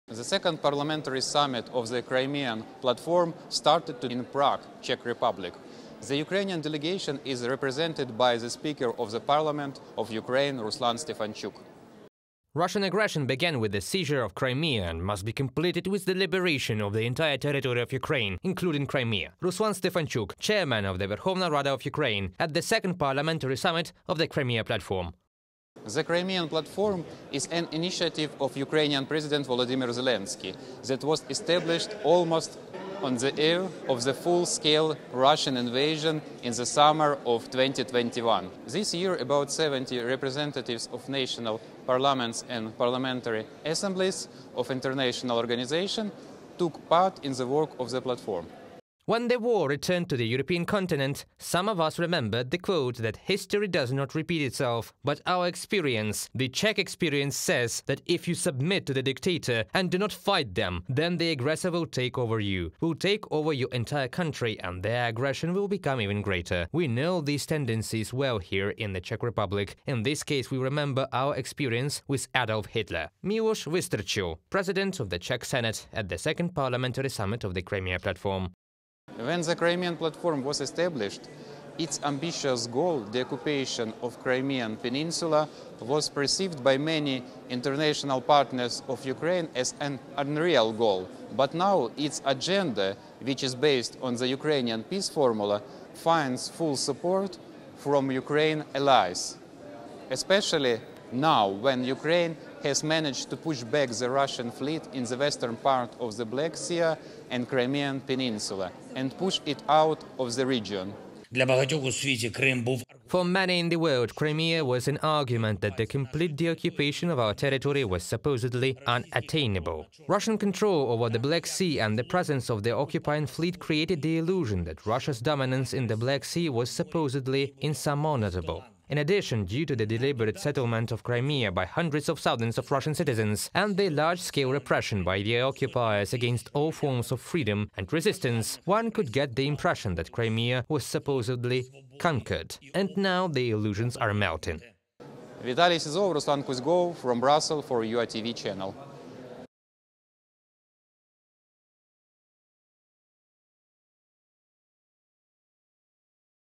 Global Support for Ukrainian Crimea. The Crimea Platform Summit
The main goal of this summit is to bring the de-occupation of Crimea closer. Current circumstances allow for the military liberation of the peninsula, but politicians work to make the civilian casualties as low as possible and drive out Russia in a political way, too. Hear more details about the summit from our special correspondent, Vitaliy Syzov.